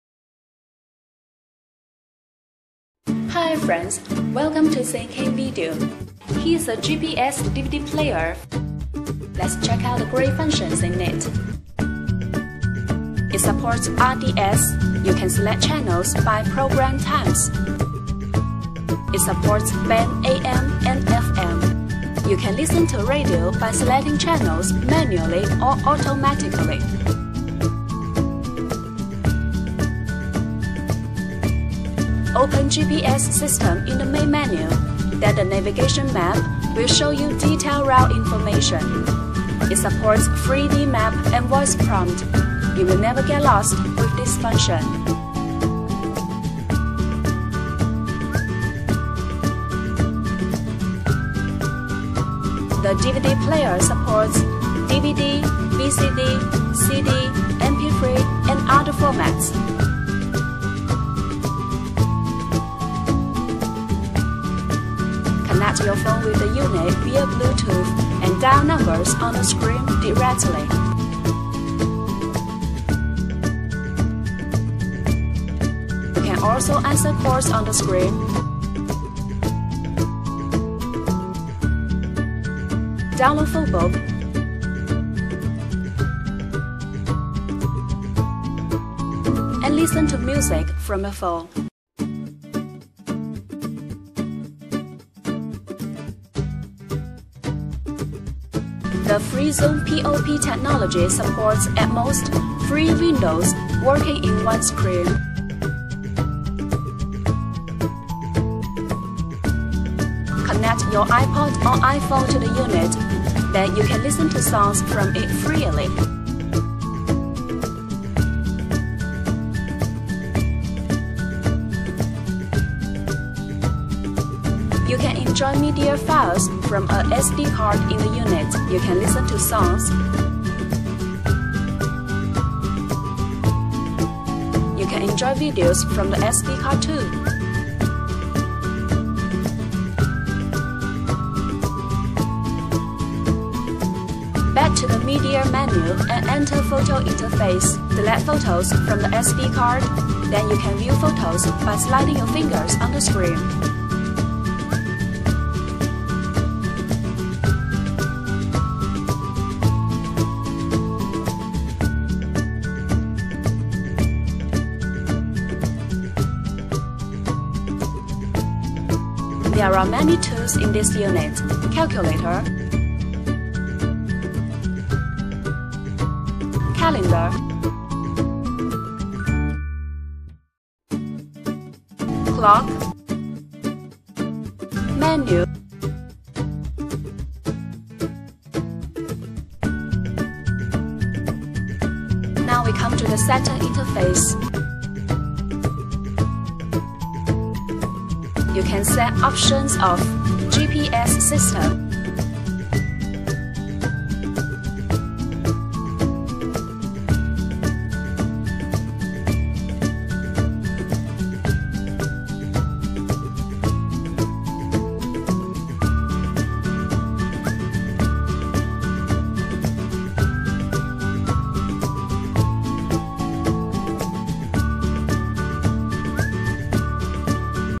Cheap 2007 2008 2009 2010 Chrysler Sebring sat nav CD DVD player support 1080P HD video play
Cheap and best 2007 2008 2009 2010 Chrysler Sebring GPS sat nav CD DVD player support 3G WIFI multimedia 1080P HD video play all TV system.
DVD function.Support DVD/DVD-R/DVD±RW/HDCD/CD-R/CD±RW/MP3/MP4/Picture CD/VCD/WMA.
Built-in 20 disk virtual CD changer.Download from CD/DVD to the head device.Support power off memory and file management to avoid keeping too many disks in the car.You can setup audio balance,high/mid/bass and play style(POP/Classical/Jazz).You can adjust the video of lightness,contrast,color and play style.